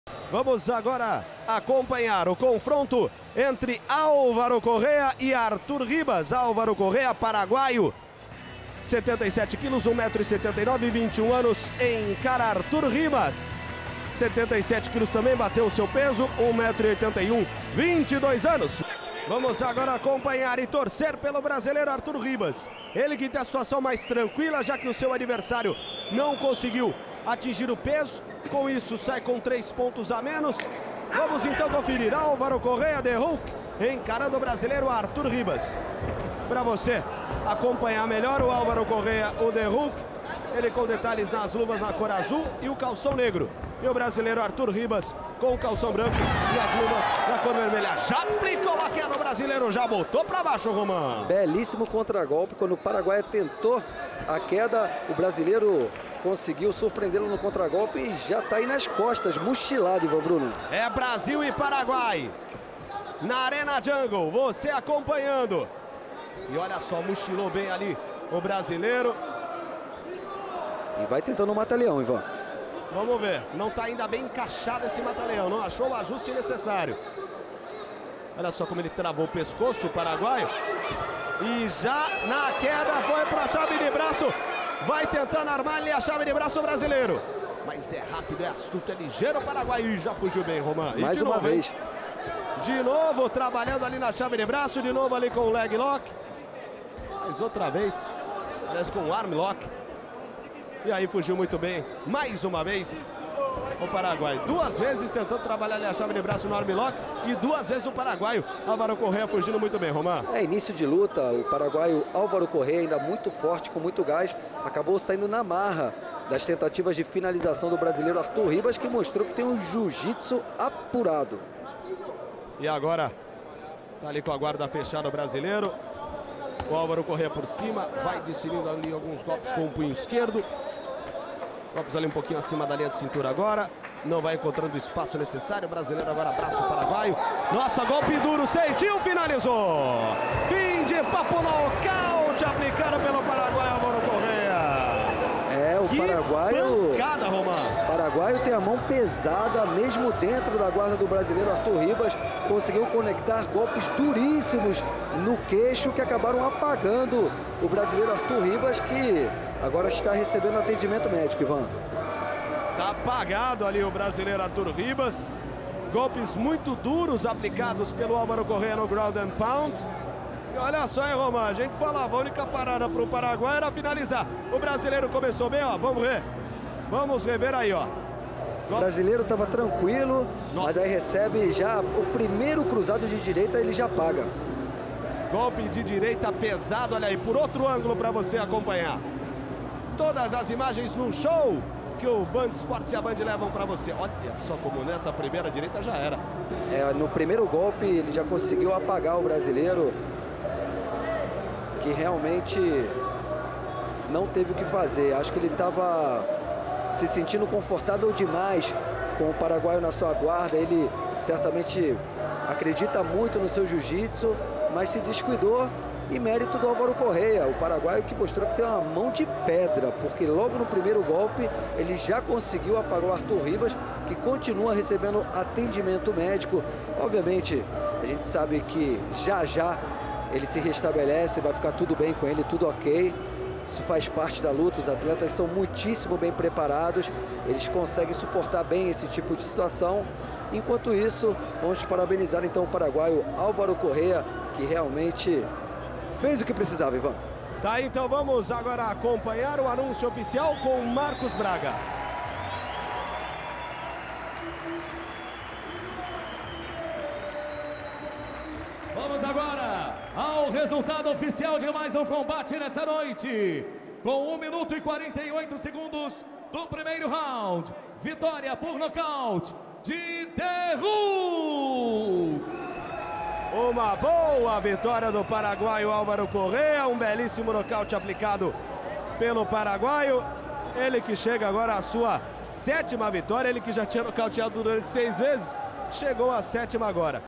JUNGLE FIGHT 75 | Alvaro Correa The Hulk x Arthur Ribas
O Jungle Fight fechou o ano com chave de ouro. A última edição de 2014 começou na noite de 18 de Dezembro varou a madrugada de sexta-feira no Ginásio da Escola Superior de Educação Física da Universidade Estadual do Pará (UEPA), na Avenida João Paulo II. As arquibancadas ficaram praticamente lotadas, e o público paraense pôde assistir os 13 atletas conterrâneos do card do JF 75.

CANAIS